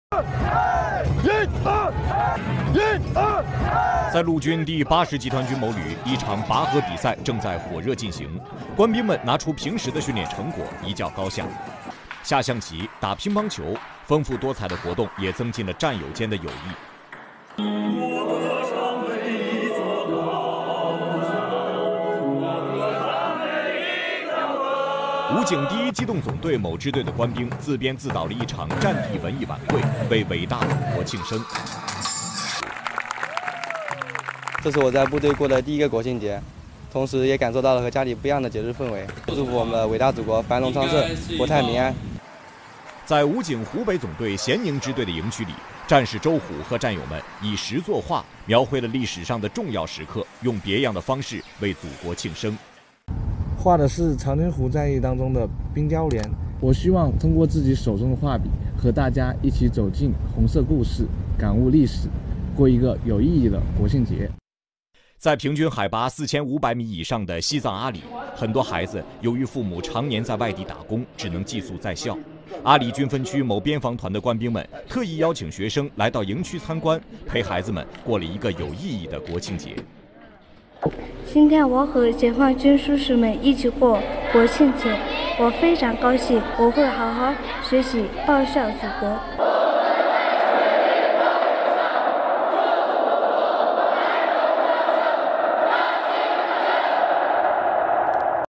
多彩文化迎国庆
节日的军营，有坚守，也有欢乐。在祖国大江南北的座座军营里，各部队举办丰富多彩的文化活动，官兵们展示特长、才艺，在欢声笑语中表达对祖国的美好祝福。